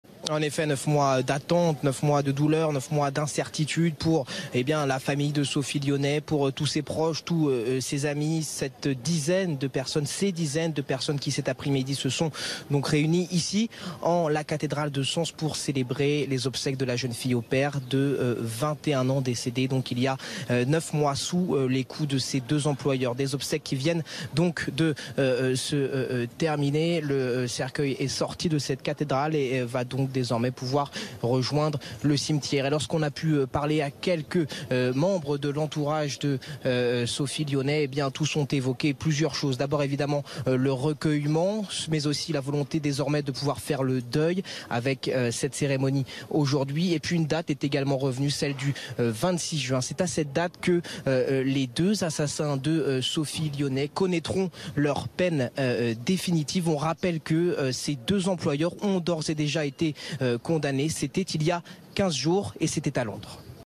Les obsèques de Sophie Lionnet viennent de s'achever
Sophie Lionnet était une jeune-fille au pair à Londres. Elle a été tuée par ses deux patrons. Ses obsèques ont eu lieu ce mercredi en la cathédrale Saint-Etienne de Sens, dans l'Yonne, neuf mois après que son corps ait été retrouvé.